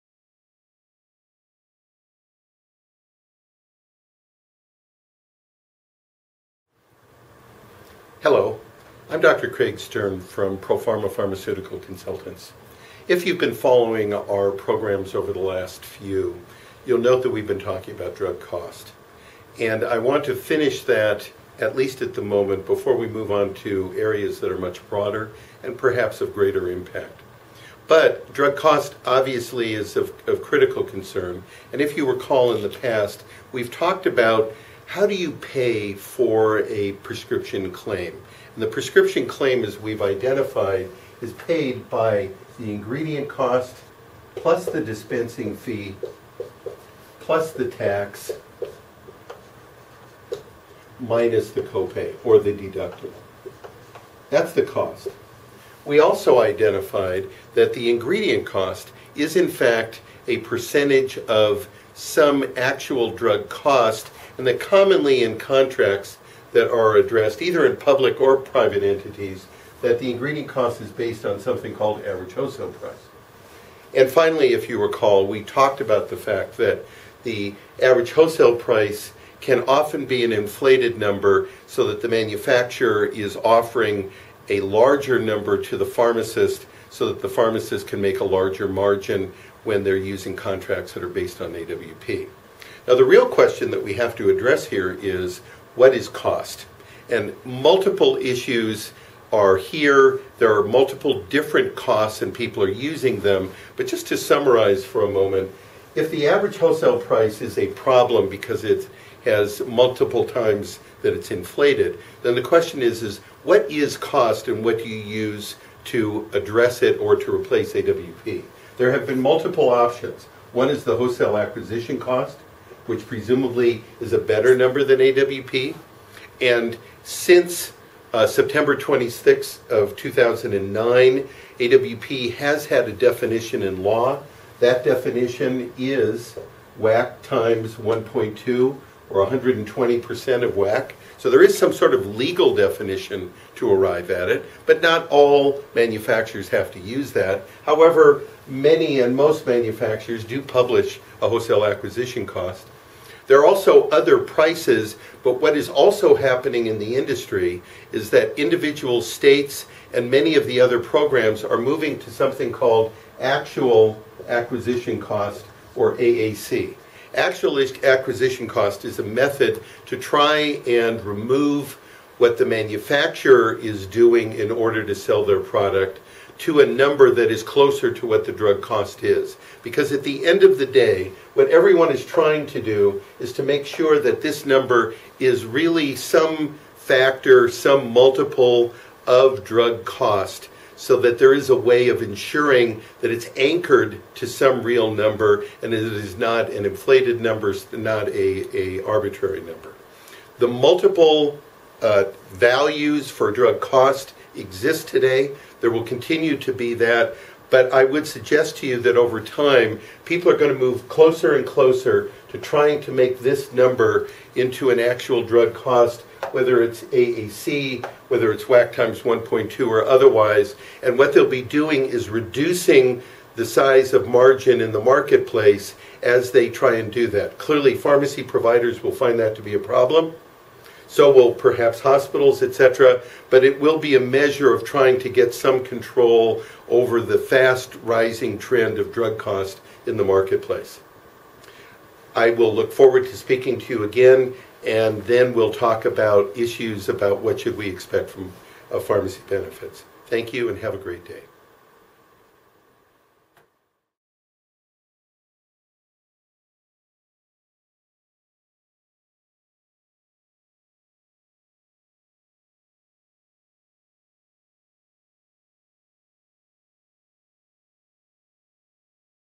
Pro Pharma: What is Drug Cost?
Drug Costs and Pricing Methodologies for increased understanding of what is occurring in the marketplace today.